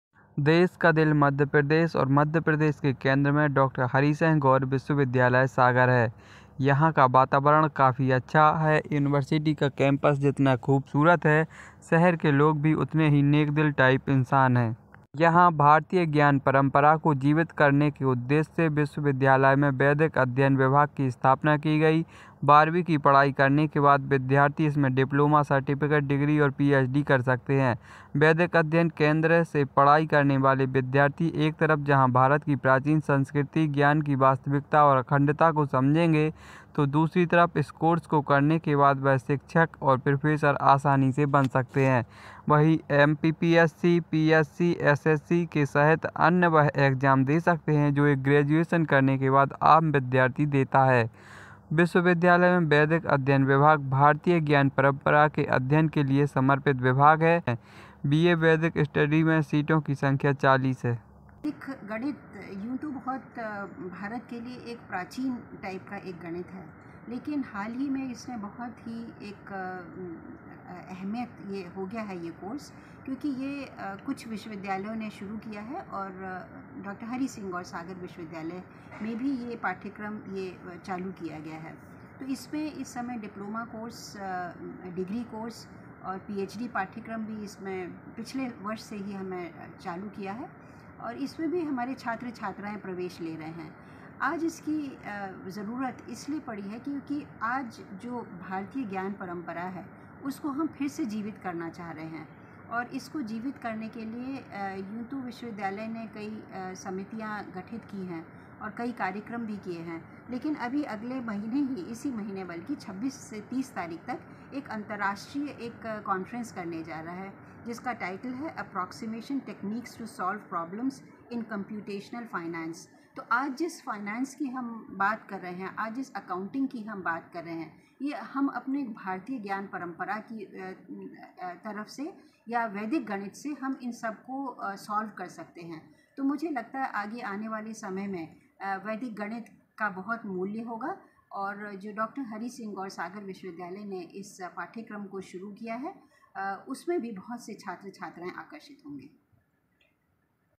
Government Job: इस यूनिवर्सिटी में अब होगी वेदों की पढ़ाई, फिर मिल जाएगी सरकारी नौकरी
Madhya Pradesh is the heart of the country and Dr. Harisingh Gour University Sagar is in the center of Madhya Pradesh. The environment here is very good, the campus of the university is as beautiful. The people of the city are also equally good-hearted people. On one hand, one can study in the university at very low fees, on the other hand, hostels and mess are also affordable here. Even in the city, living and eating is quite cheap.
देश का दिल मध्य प्रदेश और मध्य प्रदेश के केंद्र में डॉ. हरीसिंह गौर विश्वविद्यालय सागर है. यहां का वातावरण काफी अच्छा है, यूनिवर्सिटी का कैंपस जितना खूबसूरत हैं. शहर के लोग भी उतने ही नेक दिल इंसान हैं. एक तरफ जहां यूनिवर्सिटी में बेहद कम फीस में पढ़ाई हो जाती है तो दूसरी तरफ यहां पर हॉस्टल और मेस भी किफायती है. वहीं शहर में भी रहना और खाना काफी सस्ता है